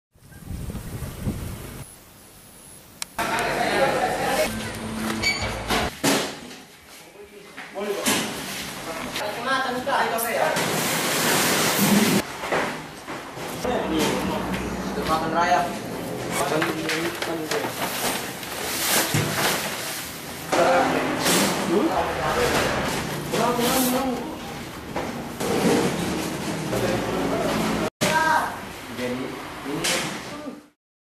32 Bilik Suara Dimakan Rayap
Metrotvnews.com, Palopo: 32 bilik suara di KPUD Kota Palopo, Sulawesi Selatan rusak dimakan rayap, sehingga tidak dapat digunakan lagi.